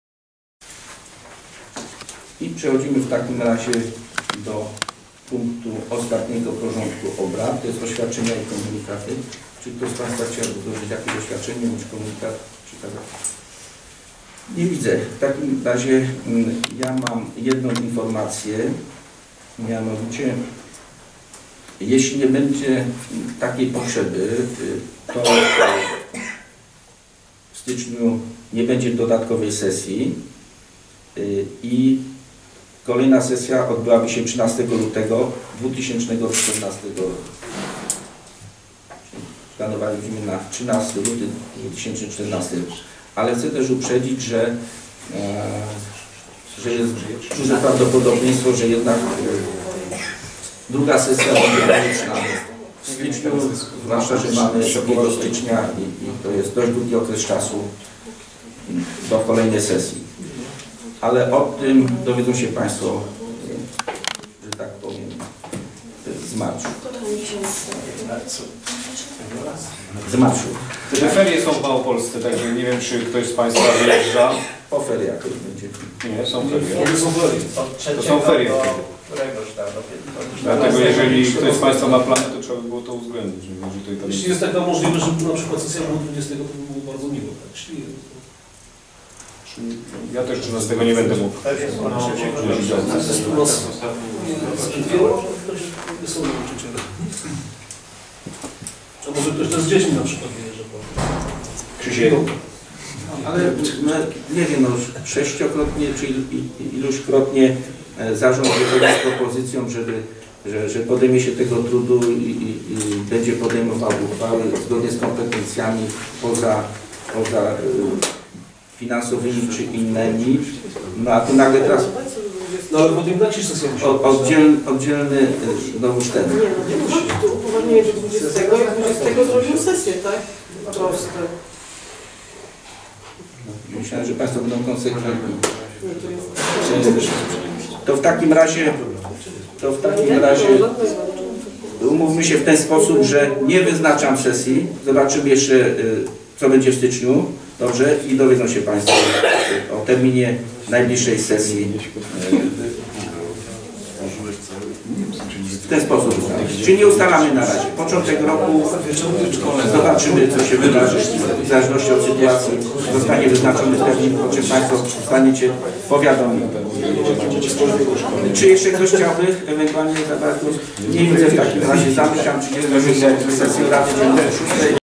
RD_VI_02-01-2014_6 oświadczenia i komunikaty
Obrady XXXVII sesji Rady Dzielnicy VI Bronowice z dn. 2 stycznia 2014 r. Oświadczenia i komunikaty.